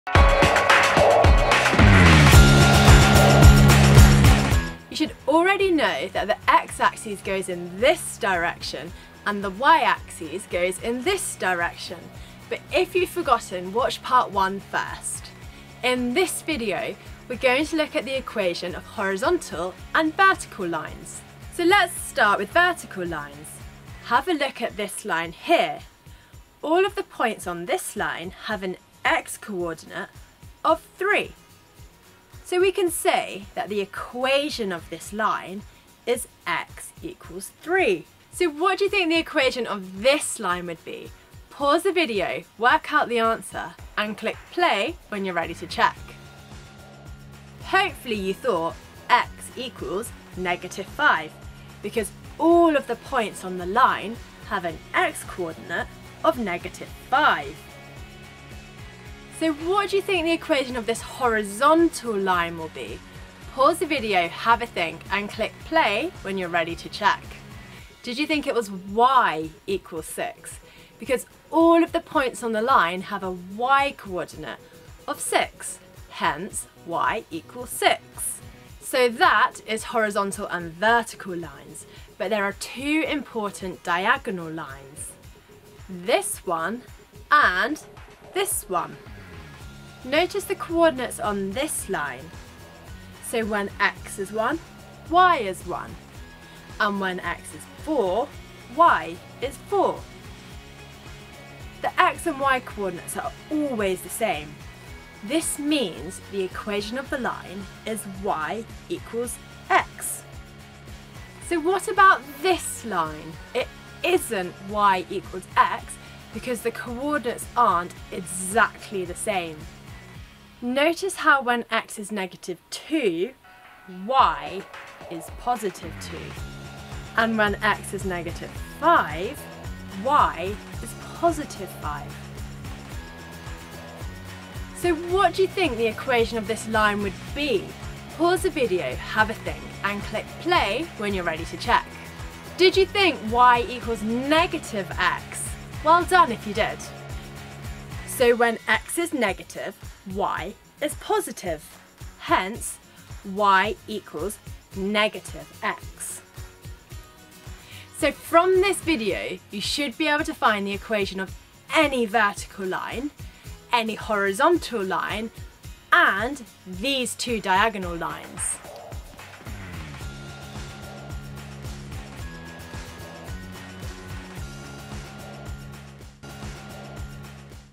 Horizontal & Vertical Lines | Graphs | Maths | FuseSchool
Learn about graphs. In this second part introductory video we will look at the equation of horizontal lines and vertical lines. We will also have a quick look at two important diagonal lines.

Vertical lines on a graph all have the same x-coordinate. This means they will have an x= equation. So x=2 and x=-3 are both vertical lines. The whole way up and down the line x=2, every single point has an x-coordinate of 2.

Horizontal lines all have the same y-coordinate. This means that they will have a y = equation. So y = 2 and y = -3 are both horizontal lines. The whole way across the y = 2 line, every single point has a y-coordinate of 2.

Diagonal lines starting from the bottom left and going up to the top right, cutting exactly through (-5, -5, (-3, -3) (0, 0) (1, 1) (3, 3) etc has the equation y = x.

Whereas the diagonal line that starts in the top left and goes down to the bottom right, cutting exactly through (-5, 5) (-3, 3) (0, 0) (3, -3) (5, -5) etc has the equation y = -x or x = -y. The numbers are the same but the sign is different.

Over the next 10 or so videos we will cover everything that you need to know about graphs, that you learn in school.